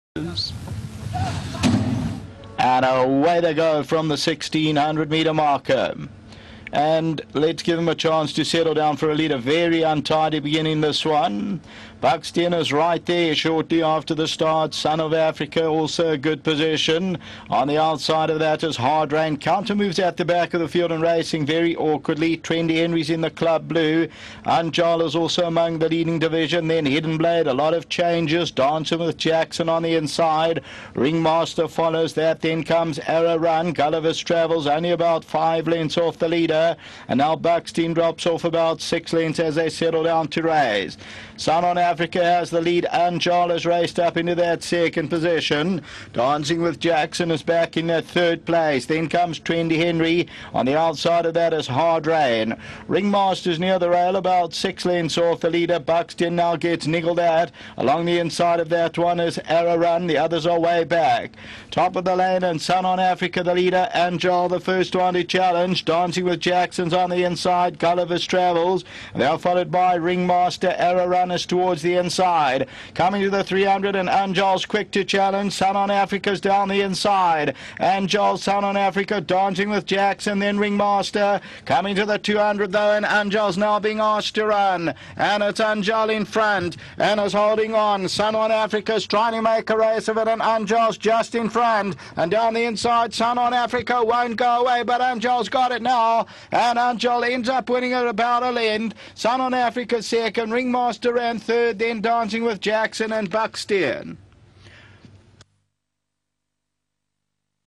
Anjaal (Lonhro x Miss Portofino)
The Mike de Kock-trained Anjaal (LOnhro x Miss Portofino breaking his maiden on 18th June, 2014 for owner Shadwell Stud. Purchased from the draft of Sledmere Stud at the Inglis Easter Yearling Sale.